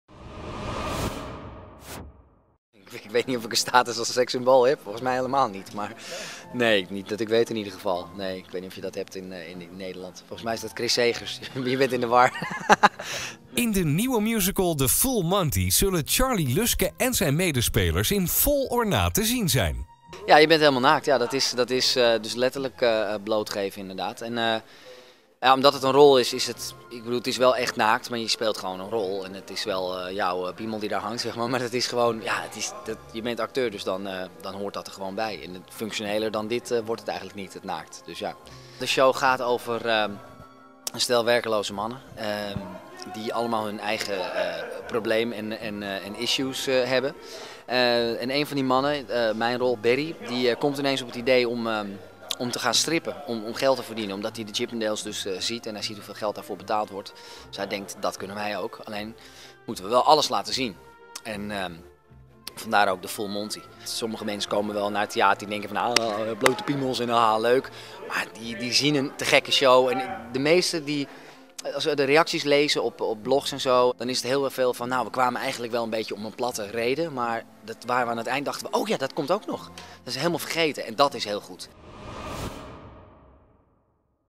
Blote billen bij première The Full Monty
Bekend Nederland was maandag massaal uitgerukt om blote billen te kijken.